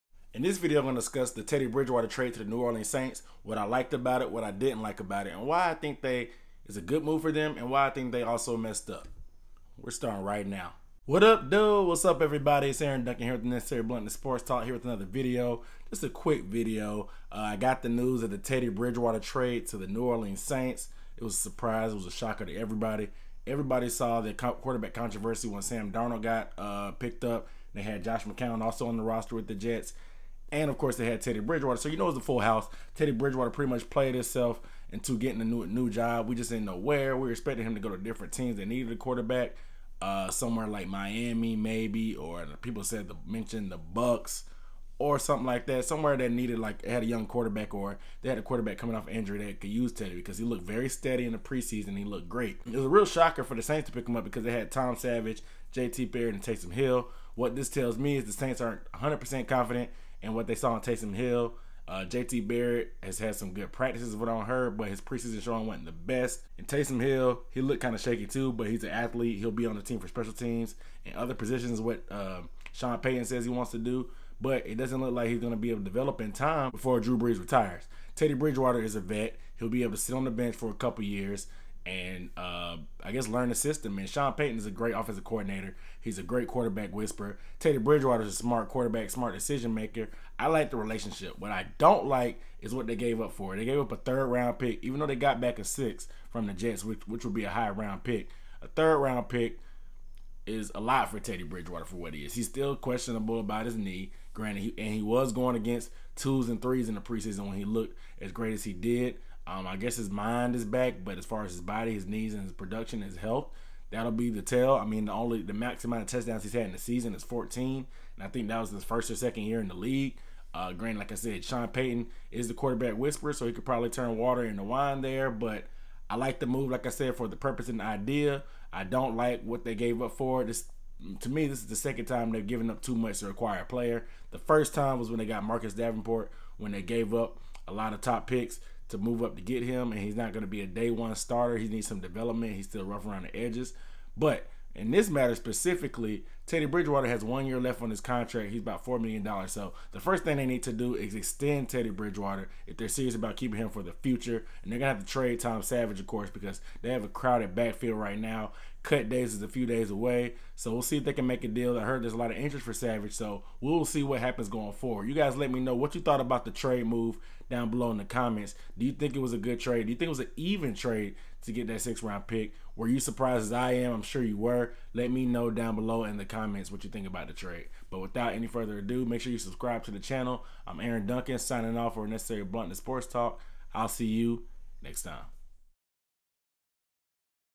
Teddy Bridgewater Traded to Saints: Love or Hate?
The news broke today that the New York Jets are trading QB Teddy Bridgewater to the New Orleans Saints for a 3rd Round Pick.  They are also receiving a 6th round pick in return.  This move came as a shocker to many around the NFL as the Saints don't have a huge need at Quarterback because of Drew Brees, but they need to look into the future.  Tune into see why I love and hate that the jets are trading teddy to the saints.